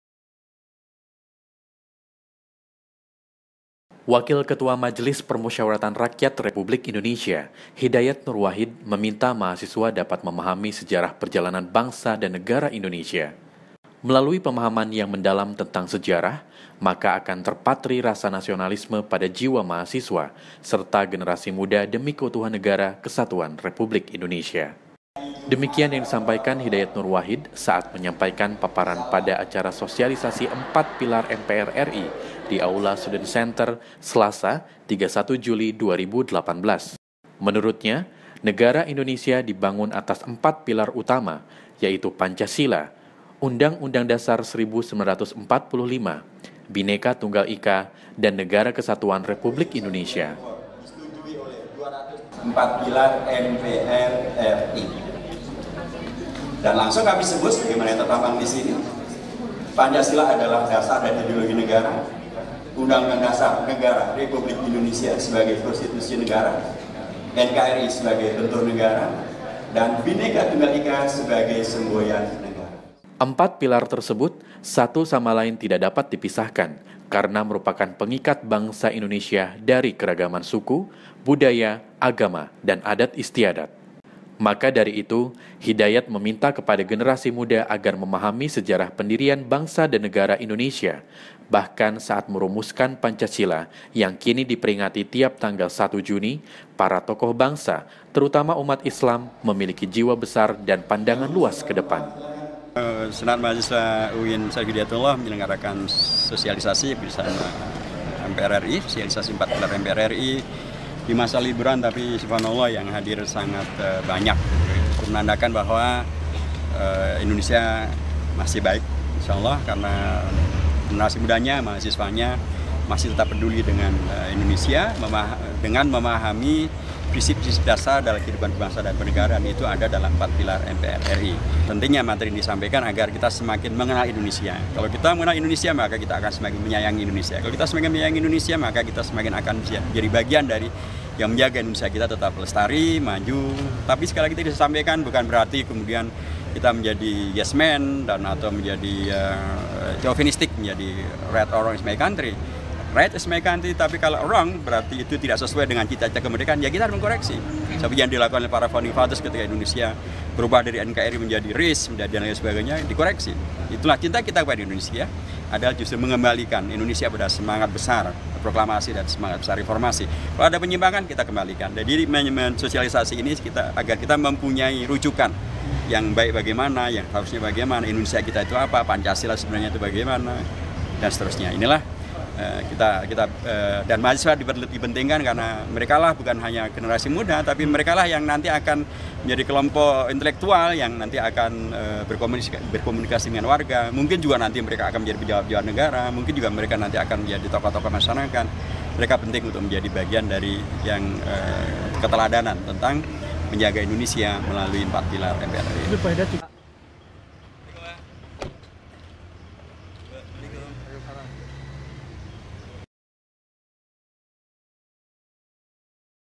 Senat Mahasiswa Undang Hidayat Nur Wahid Sosialisasikan Empat Pilar MPR RI
Wakil Ketua Majelis Permusyawaratan Rakyat Republik Indonesia, Hidayat Nur Wahid meminta mahasiswa dapat memahami sejarah perjalanan bangsa dan negara Indonesia. Melalui pemahaman yang mendalam tentang sejarah maka akan tercipta rasa nasionalisme pada jiwa mahasiswa serta generasi muda demi keutuhan Negara Kesatuan Republik Indonesia.

Demikian yang disampaikan Hidayat Nur Wahid saat menyampaikan paparan pada acara Sosialisasi Empat Pilar MPR RI di Aula Student Center, Selasa (31/7).

Pubdok UIN Syarif Hidayatullah Jakarta